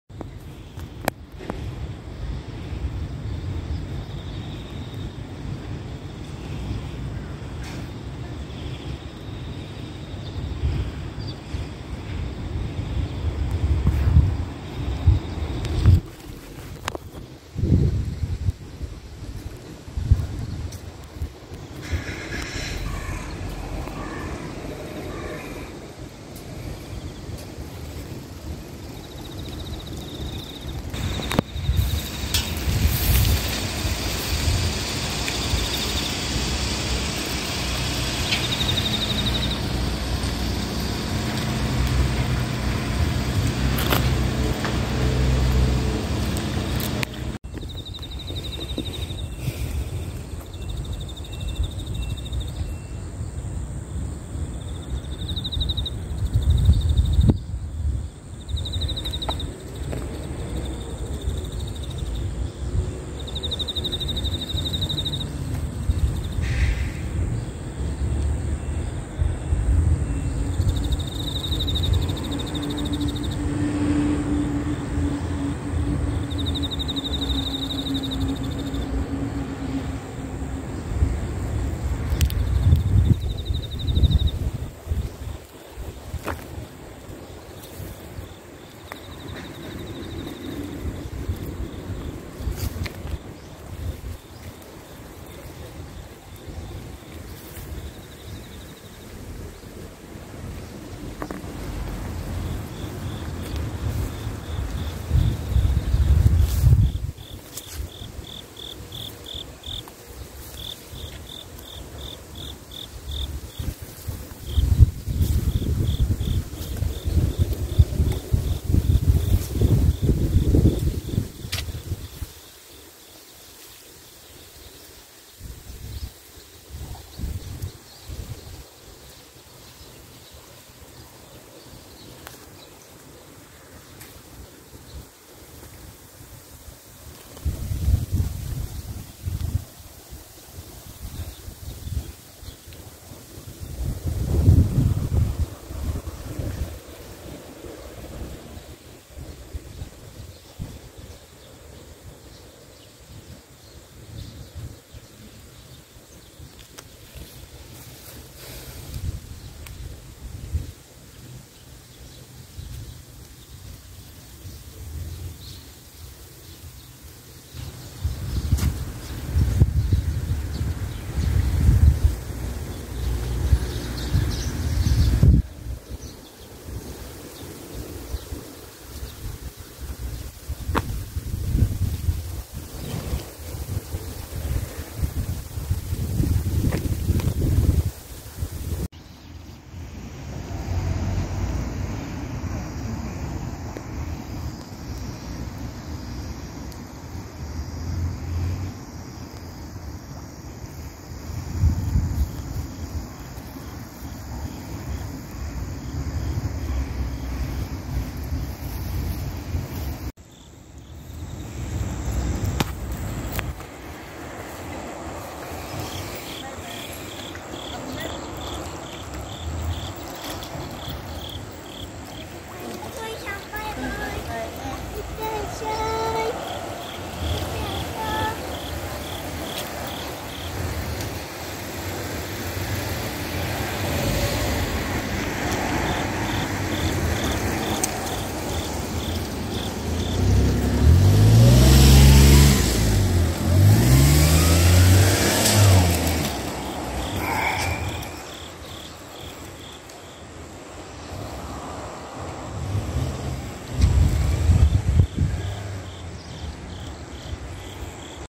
Koi Fish in Drainage Canal, Kurume
There are numerous koi fish found in the drainage canal in Kurume. As you approach, they swim toward you hoping that you would feed them.